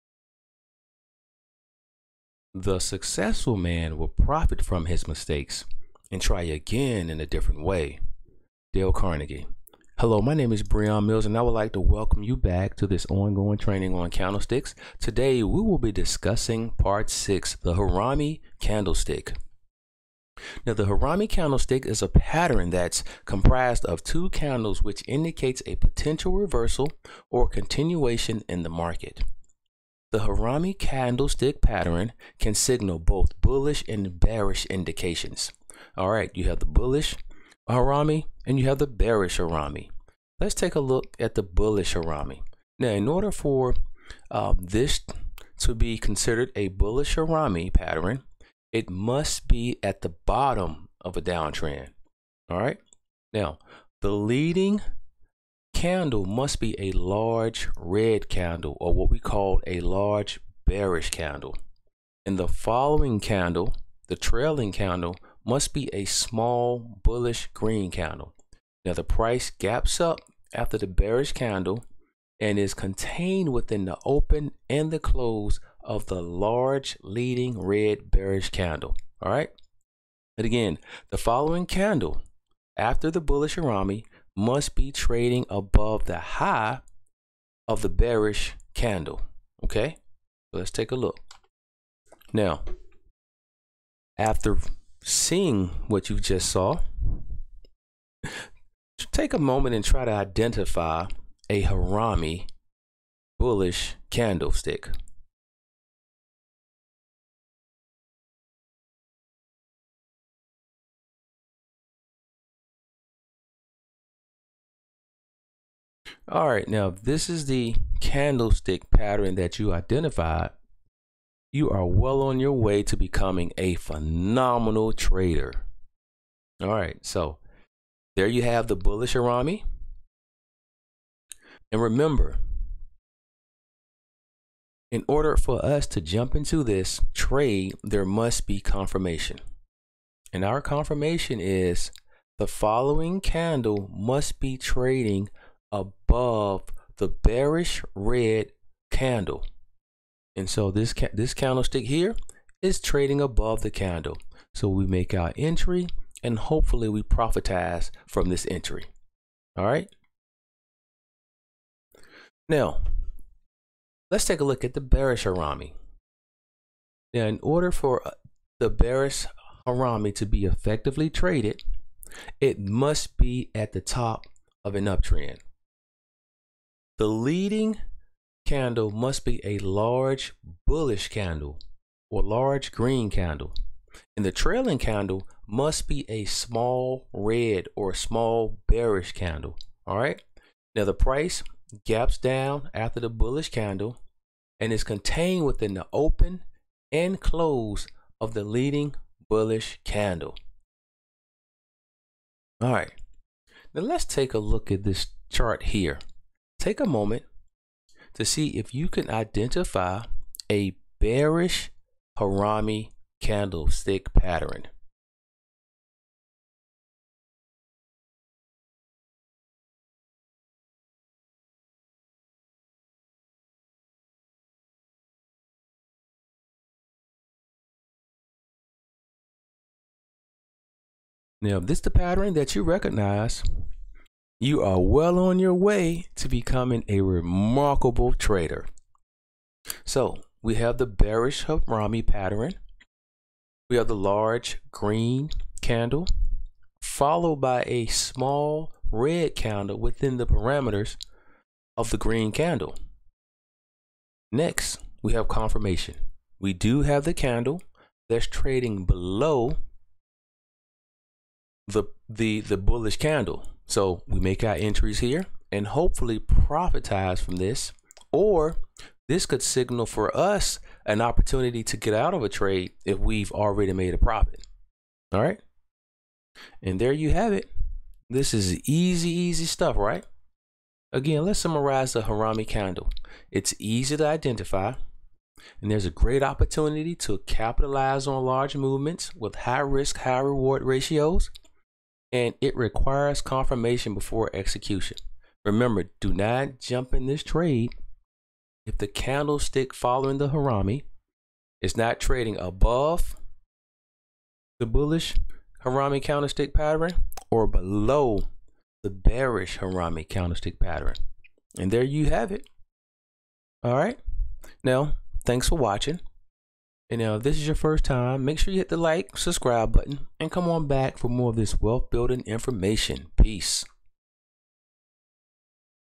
Candlestick Trading Course Part-6 The Harami
This presentation is a simplified, easy to understand presentation on candlesticks. You will walk away from this presentation with a better understanding of candlesticks and trading strategies you can use to create wealth.